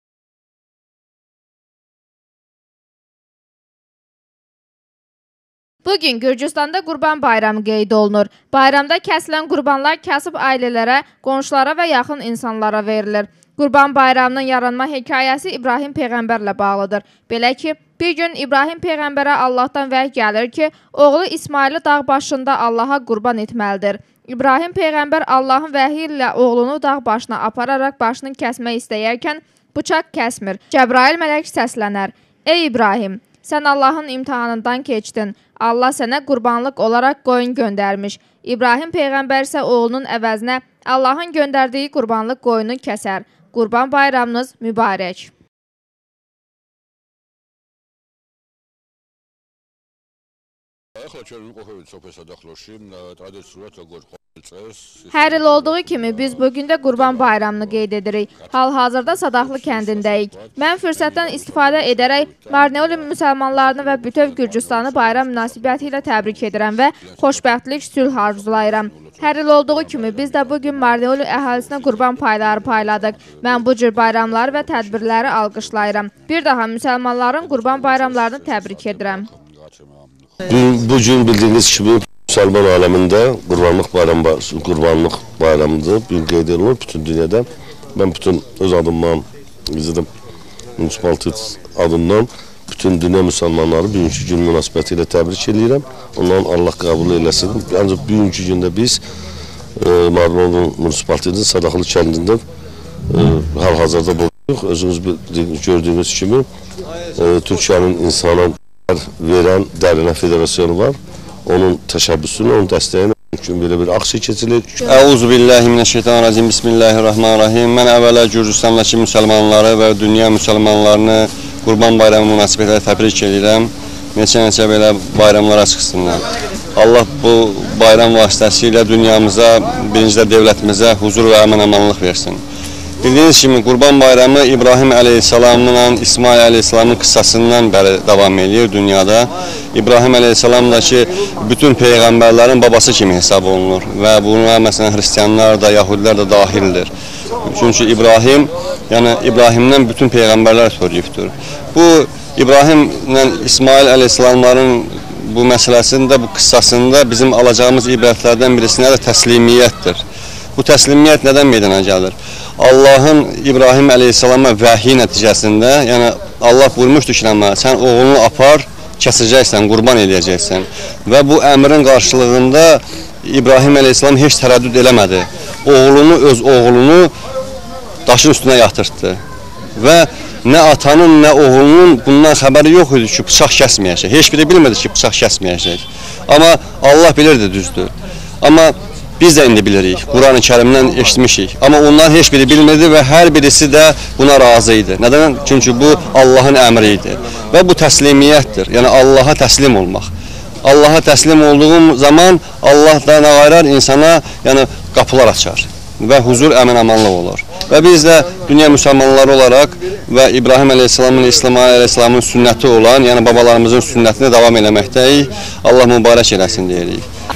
Marneuli Bələdiyyəsində Qurban Bayramı Marneuli TV
Marneuli Bələdiyyəsində Qurban Bayramı qeyd olundu